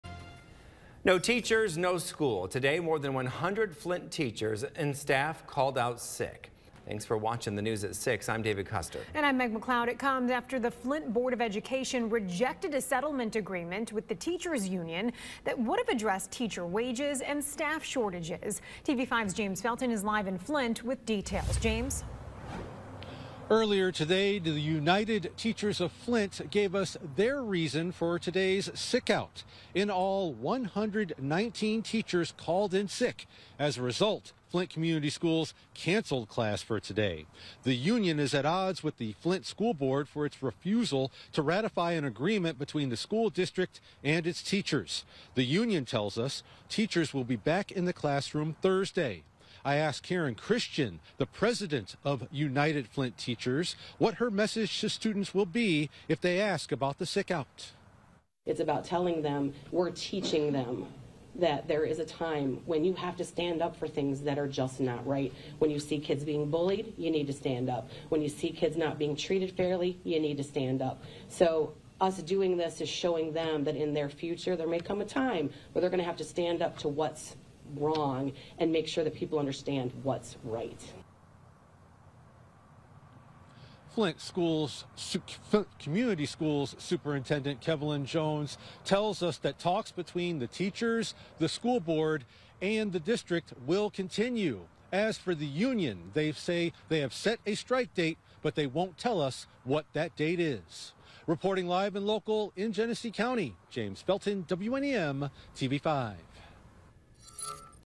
‘These teachers have had enough’: Over 100 Flint teachers call out on Wednesday
More than 100 Flint teachers and staff called out sick on Wednesday, causing schools in the district to close for the day.